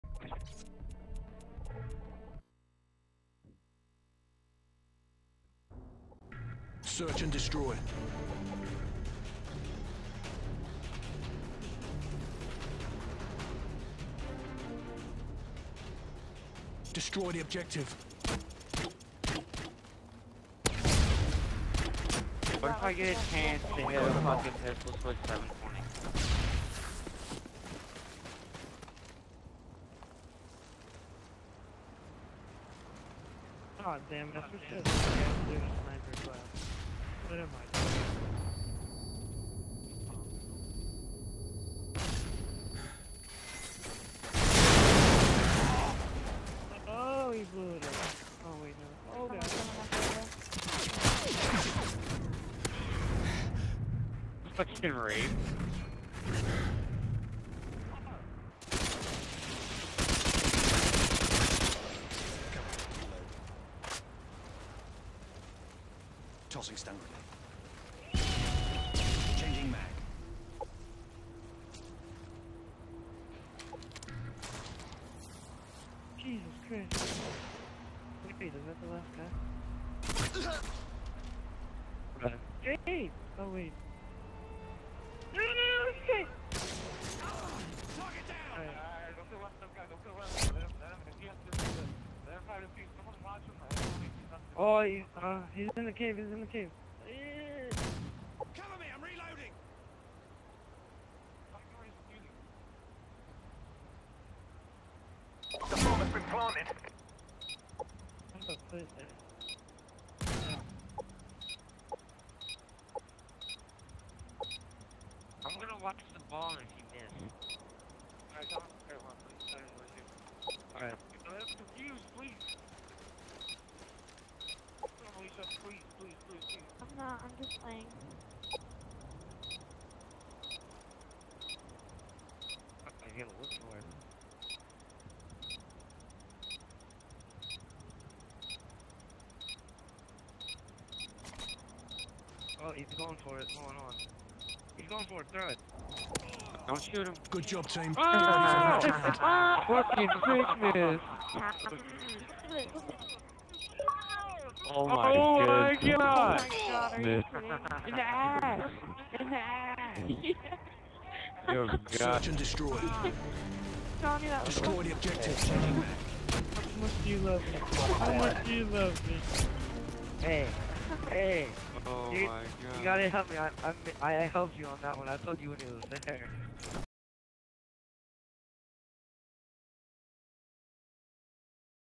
insane throwing knife
this is an insane kill cam throwing knife across afghan me and all QSP members took a while to get it done but we finally got it down with teamwork SUBSCRIBE